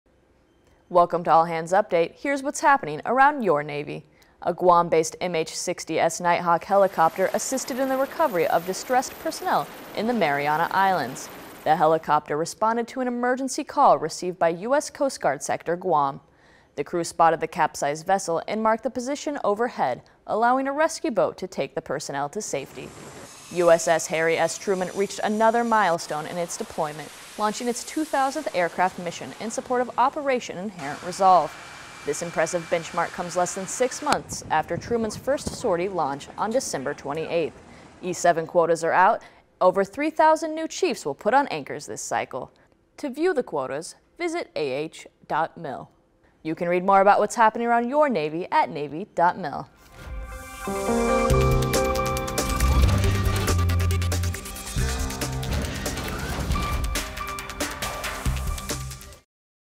Headlines for Tuesday, June 21, 2016
MH60S Knighthawk assisted in the recovery of distressed personnel in the Mariana Islands. USS Harry S. Truman launched its 2000th sortie. E-7 quotas were released.